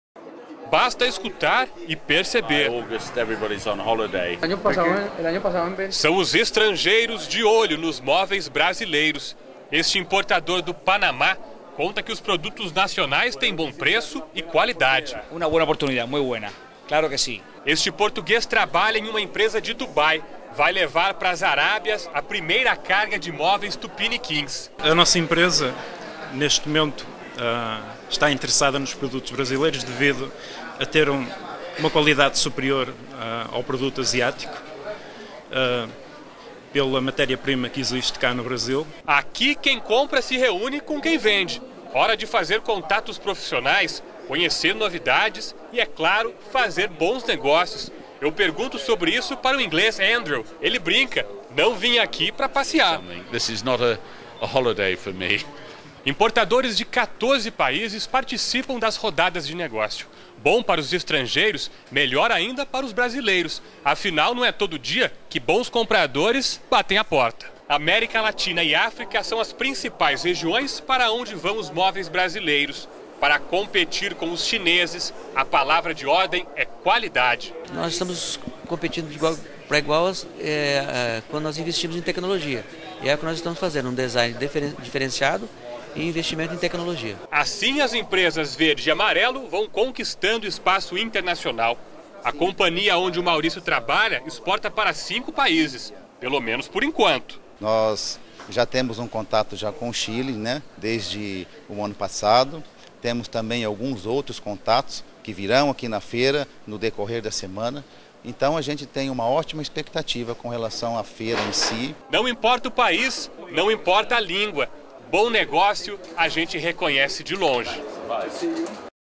Importadores de olho nos móveis brasileiros - Movelpar Arapongas
A Movelpar, Feira de Móveis do Paraná, é uma vitrine de produtos para o mercado exterior. Importadores de 14 países se reuniram em Arapongas, norte do Paraná, para fechar negócios.

Reportagem: Vinicius Frigeri
Imagens: Leandro Longo

RICTV Record
11/03/2013